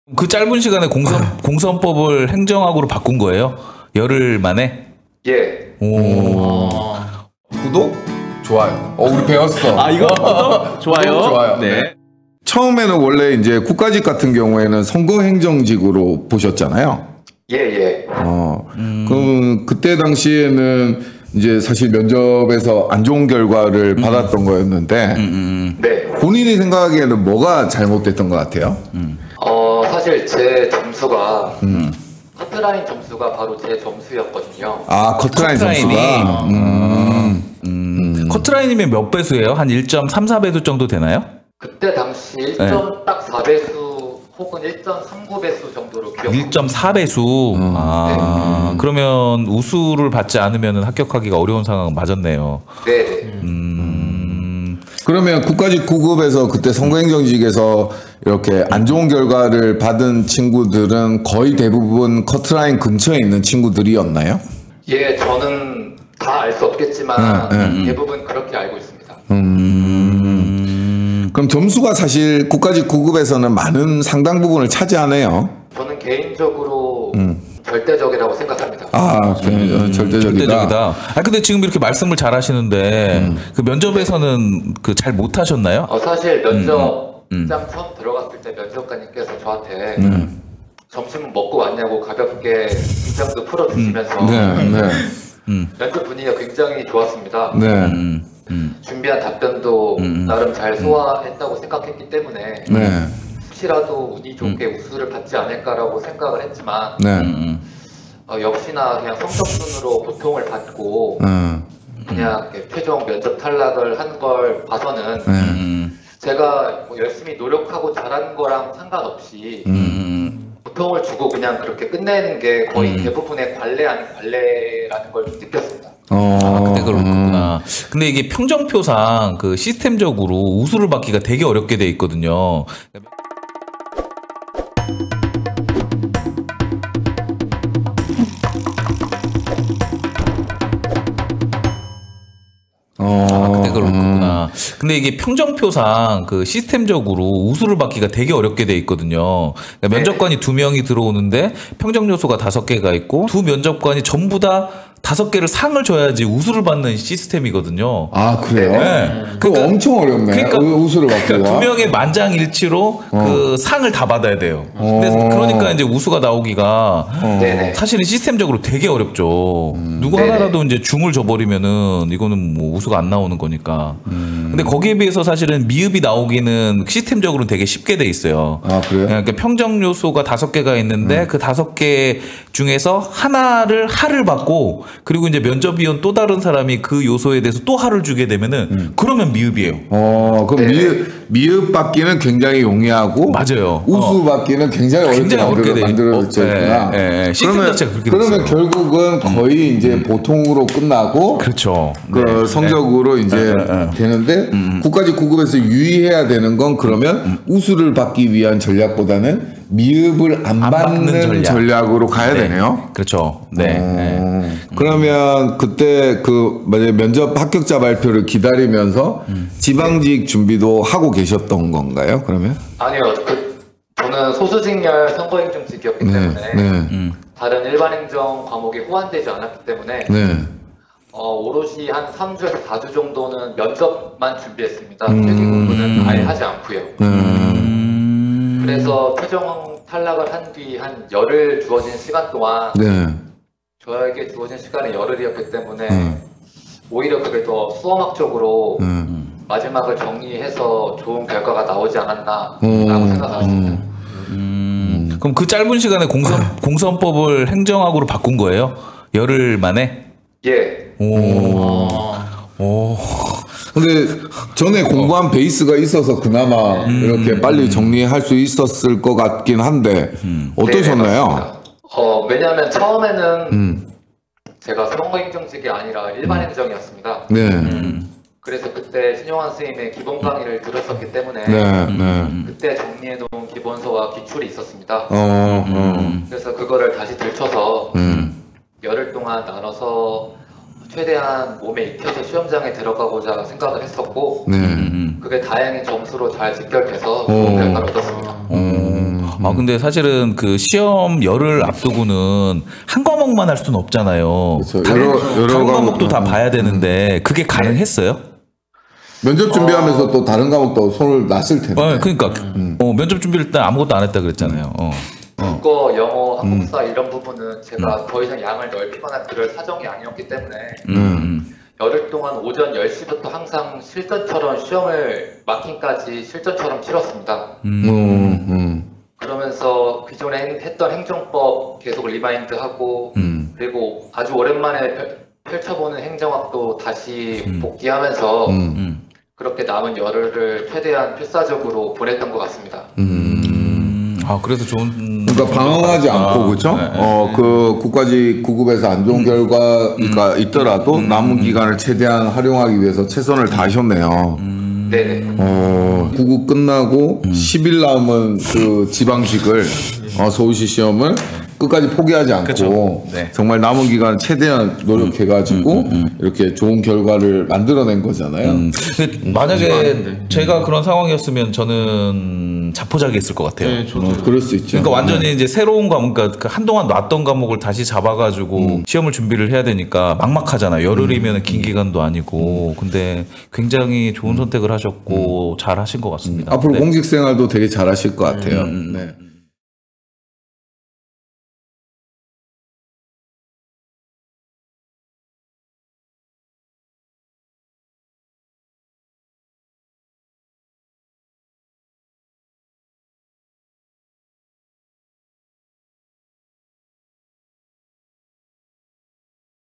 공무원시험 국가직9급 면접 탈락 후 10일 만에 지방직 합격한 현직자 인터뷰!
공시생의 합격메이트, 공무원 정보 유튜브 난공불락TV!